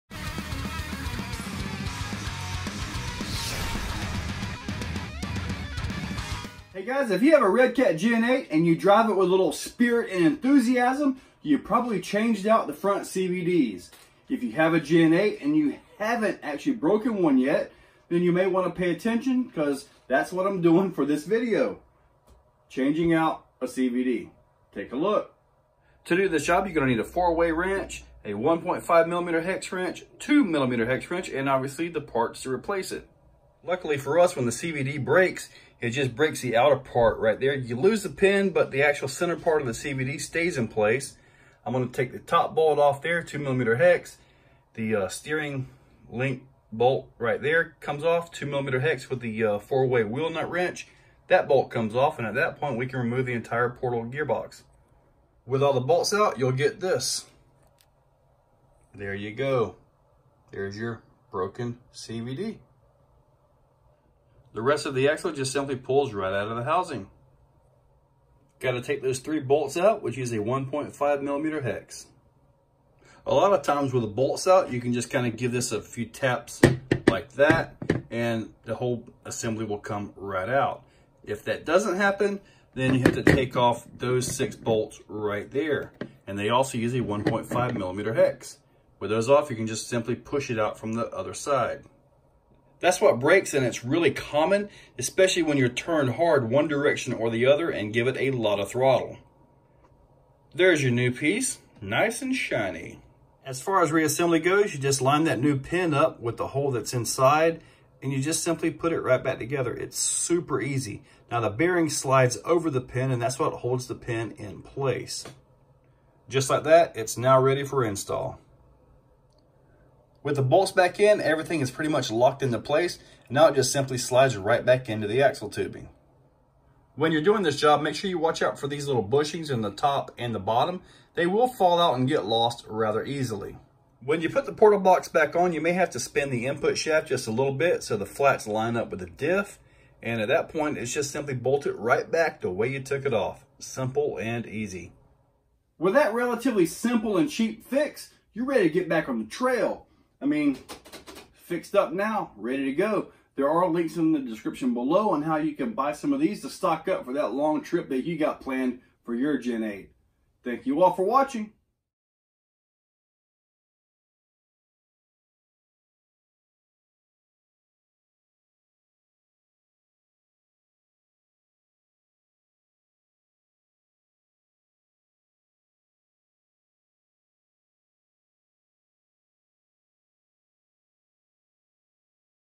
Gen8 CVD Change How to
Not a hard job at all, just wish they were stronger, or we had stronger options.

Replacement CVD's

Hot Racing lower link mounts

Redcat Lower link mounts

Redcat Chubs

All kinds of Aluminum Redcat stuff